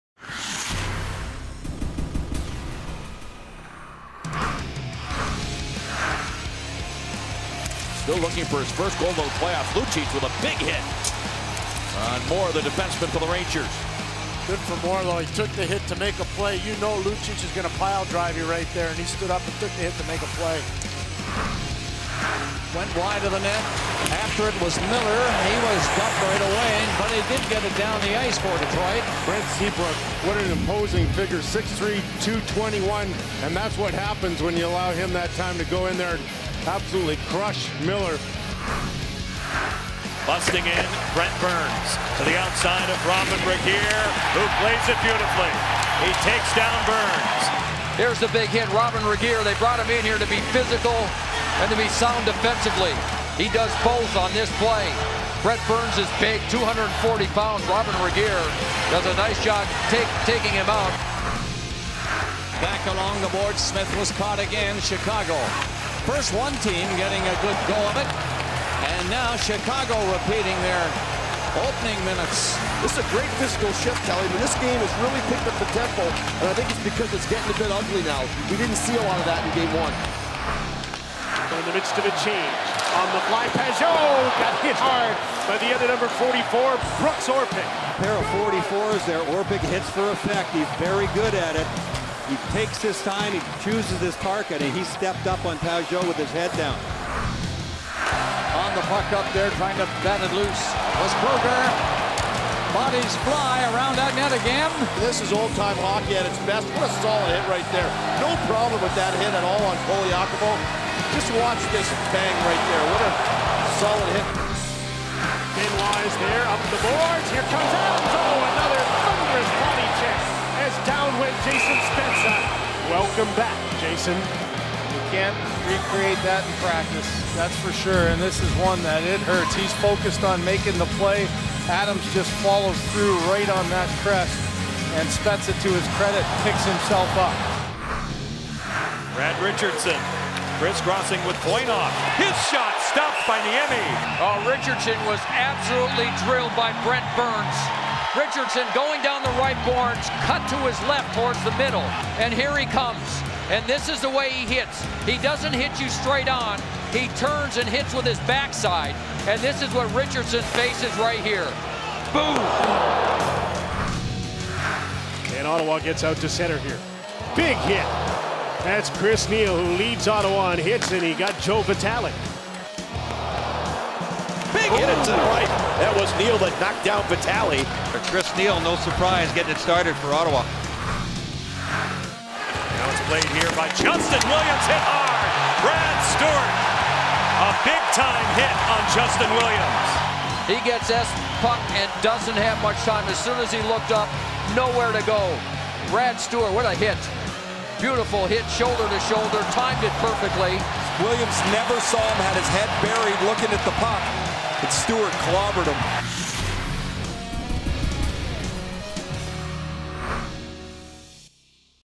Top 10 Hits of the Week 05/24/13
The greatest hits from the past week!!! Enjoying!!!
10. Milan Lucic
9. Brent Seabrook
8. Robin Regehr
7. Brendan Smith
6. Brooks Orpik
5. Andrew Shaw
4. Craig Adams
3. Brent Burns
2. Chris Neil
1. Brad Stuart